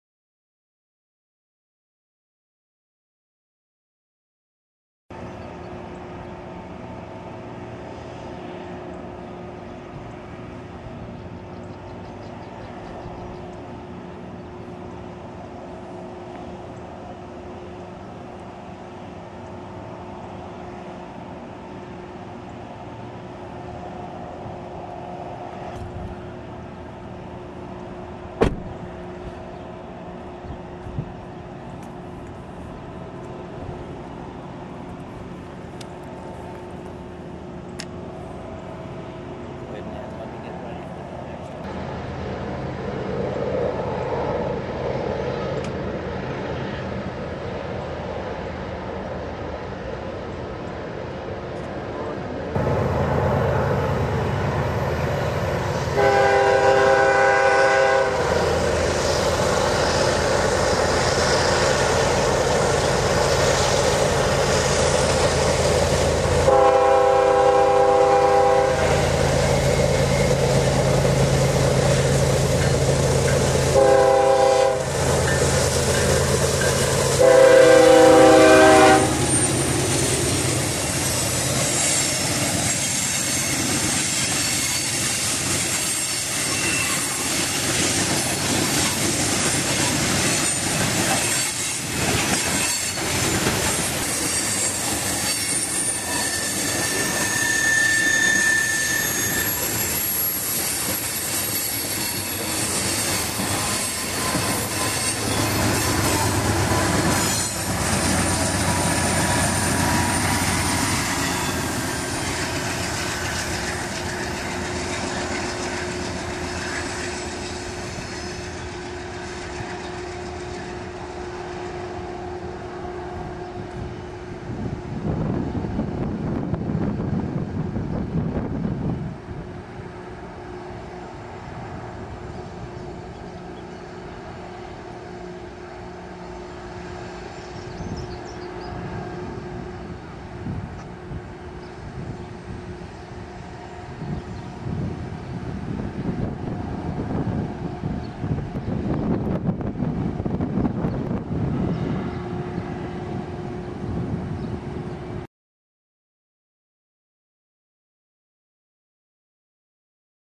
BNSF 7650 WB at Sibley, MO
BNSF 7650 WB coming off the Missouri river bridge at Sibley 5-25-09